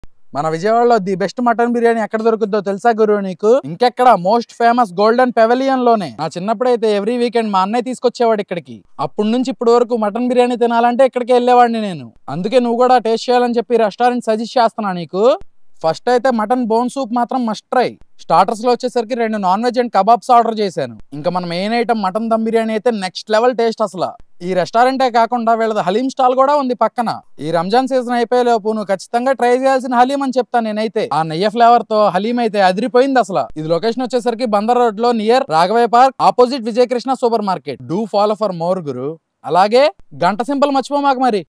Best mutton biryani in Vijayawada || Golden pavilion restaurant vijayawada food foodchallenge ￼
Best mutton biryani 😍
Do follow for more Guru 🔔

Dragan chi-390
Malia kabab-400
Slice chi-330
Mutton bone soup-180
Mutton biryani-460

Haleem 300
Spl haleem 400
Haleem family pack 650
Address

27-12-39 Bandar Road Near Raghavaiah Park, Opposite Vijaya Krishna Super Bazar, Vijayawada, Andhra Pradesh 520002